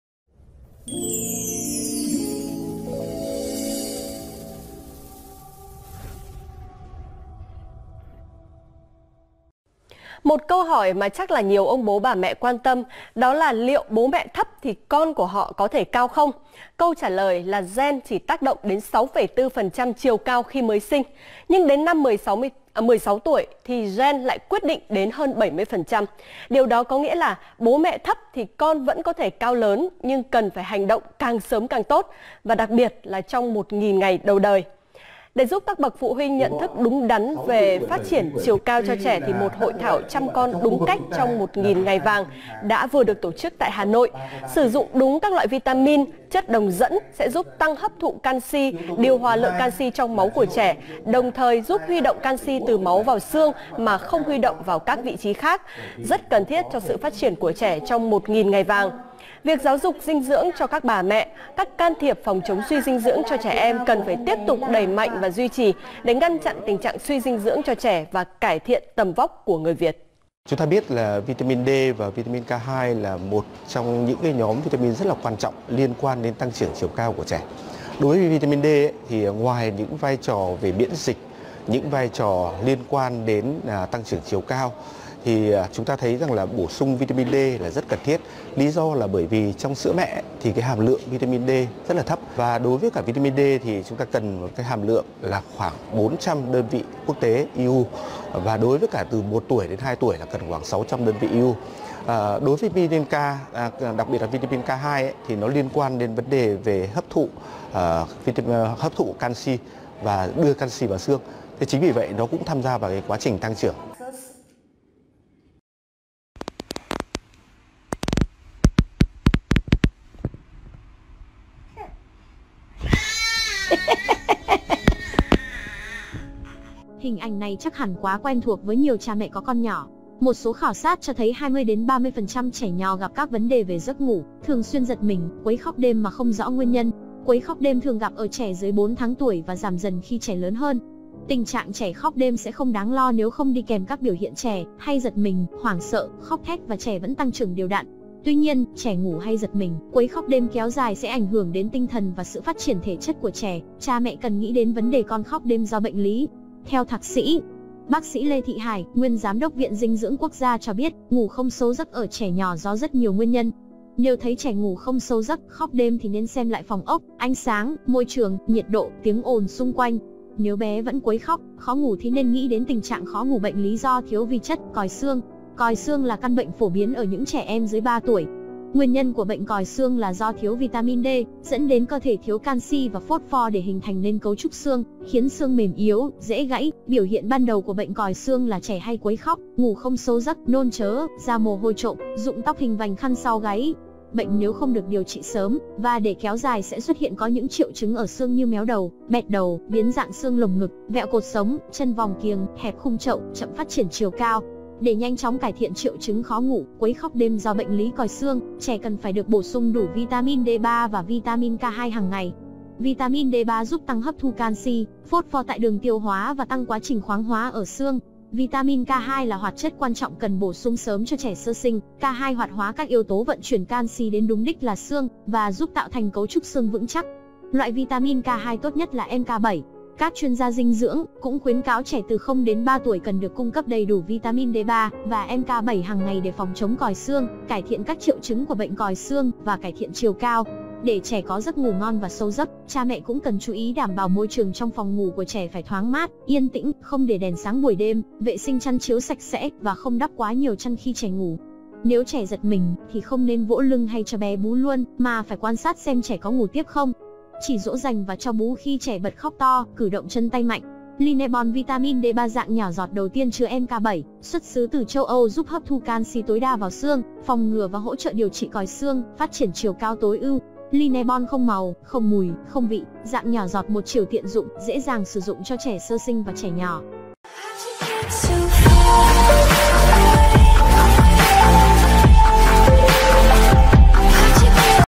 Vitamin D3 K2 MK7 LineaBon giúp trẻ hết khóc đêm, phát triển chiều cao
MẸ ƠI! ĐỪNG SỢ CON LÙN
CHĂM CON ĐÚNG CÁCH 1000 NGÀY VÀNG LÀ CAO

Bố mẹ thấp nhưng con hoàn toàn có thể cao chuẩn nếu can thiệp dinh dưỡng càng sớm càng tốt, đặc biệt giai đoạn 1000 ngày đầu đời. Bởi gen chỉ quyết định 6,4% chiều cao khi mới sinh, cho đến khi 16 tuổi gen lại quyết định đến 71,8% chiều cao của trẻ.
Đặc biệt, việc bổ sung vitamin D3 và K2 là rất cần thiết giúp tăng hấp thụ canxi, điều hòa canxi trong máu của trẻ, đồng thời giúp huy động canxi từ máu vào xương mà không huy động vào vị trí khác, cải thiện mật độ xương, giúp trẻ tăng chiều cao vượt trội.

HÃY BỔ SUNG NGAY LINEABON D3 K2- BỘ ĐÔI TĂNG CHIỀU CAO ĐẦU TIÊN TỪ CHÂU ÂU TẠI VIỆT NAM
Lineabon là sản phẩm D3K2 HÀNG ĐẦU Việt Nam đã được các chuyên gia dinh dưỡng đầu ngành khuyên dùng và an toàn tuyệt đối cho trẻ sơ sinh và trẻ nhỏ bởi những tính năng đặc biệt:
+ Hấp thu tối đa canxi, phát triển chiều cao vượt trội
+ Đây là sản phẩm chứa bộ đôi vàng vitamin D3 400 IU và vitamin K2 (MK7) 22,5mcg đây là liều lượng đạt tiêu chuẩn giúp trẻ phát triển chiều cao tối đa. Bộ đôi vitamin này giúp Canxi đến đúng đích, loại bỏ nhanh chứng ngủ không sâu giấc đổ mồ hôi trộm, rụng tóc, còi xương, tăng chiều cao tối đa cho trẻ sơ sinh và trẻ nhỏ.
+ Chứa K2 & OliveTM là loại K2 có hoạt tính sinh học cao nhất
+ Lineabon D3K2 chứa duy nhất thành phần K2&olive, loại vitamin K2 tự nhiên duy nhất có nguồn gốc từ chủng Bacillus subtilis natto, được cấp bằng độc quyền sáng chế về các công nghệ lên men sinh học, không sử dụng các hóa chất và dung môi.
+ Khác với K2 thông thường, đây là vitamin K2 tinh khiết nhất đã được chứng minh lâm sàng có hoạt tính sinh học và độ ổn định cao nhất trong cơ thể, giúp tăng hiệu quả hấp thu canxi đến xương, phát triển chiều cao tối đa cho trẻ.
+ Không màu, không mùi, dễ uống
+ Lineabon D3K2 thiết kế đặc biệt dung dịch nhỏ giọt 1 chiều không màu, không mùi vị, không chứa chất bảo quản, lactose, kim loại, GMO cho liều chính xác, bé rất dễ hấp thu, dễ uống, không sợ nôn trớ, thích hợp cho cả những bé có cơ địa khó hấp thu.
*SẢN PHẨM KHÔNG PHẢI LÀ THUỐC – KHÔNG CÓ TÁC DỤNG THAY THẾ THUỐC CHỮA BỆNH*